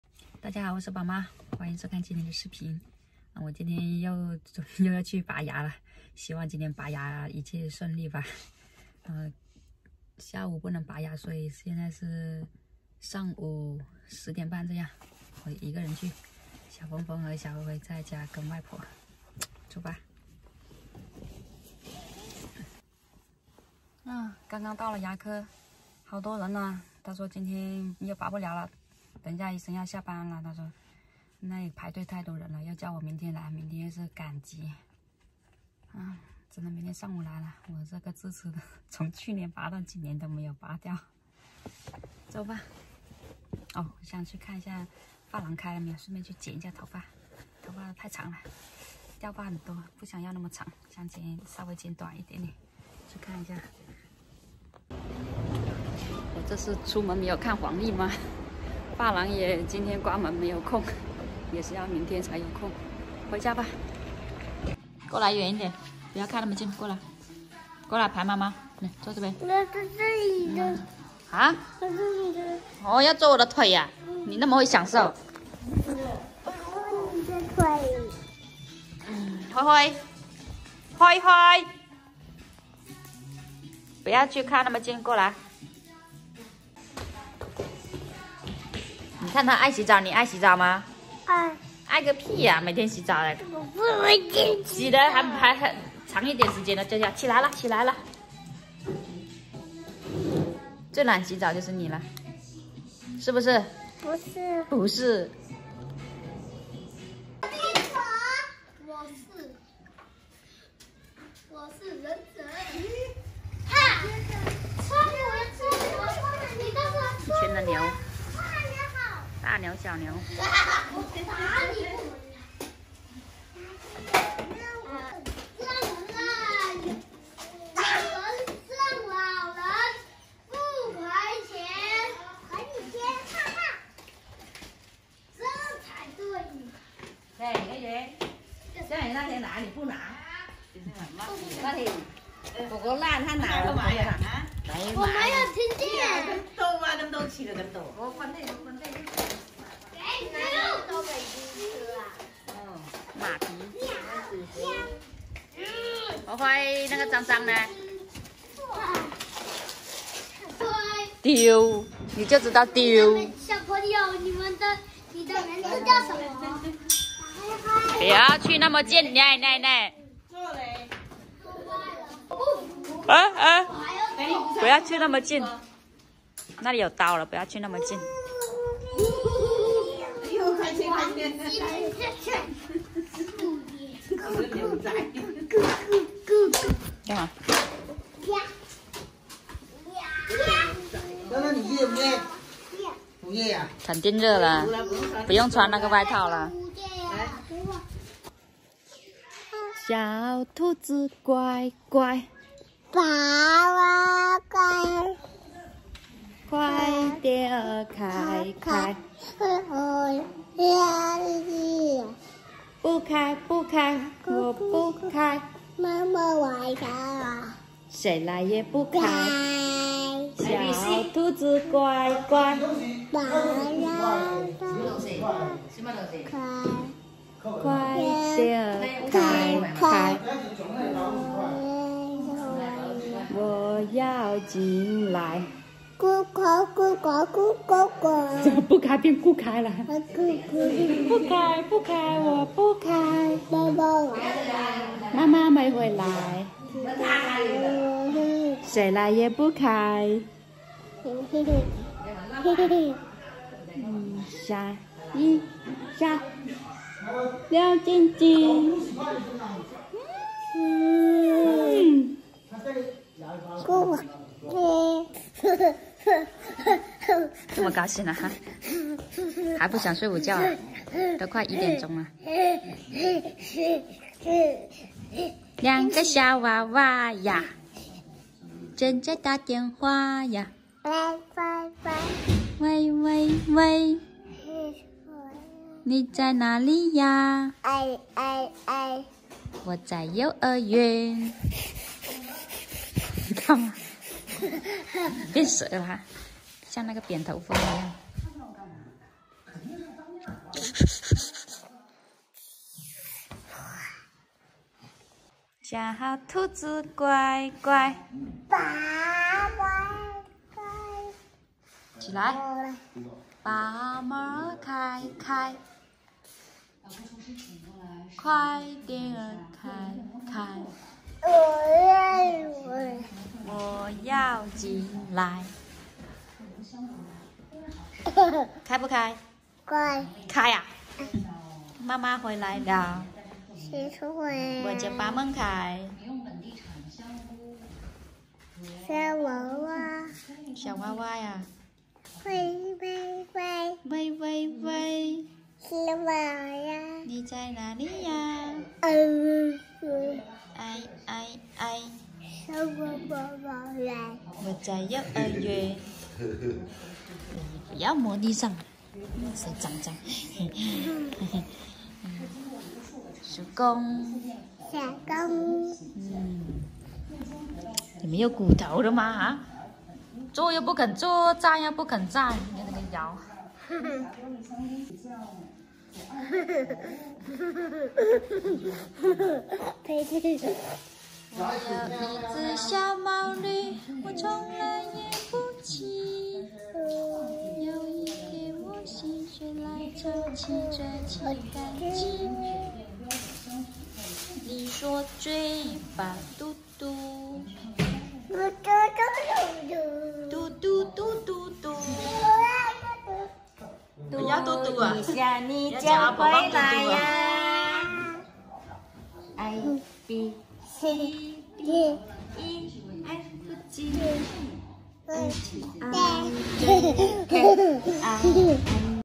歌星辉又上线啦！萌萌的小奶音，让人百听不厌，还学了新歌哦！【小枫枫和小辉辉】
小辉辉展歌喉，萌萌的唱着童歌，真是太可爱了~
记录农村宝宝成长生活，开心快乐与您分享！ 我们强调了视频内容的合规性，绝对没有任何不良引导或危险行为。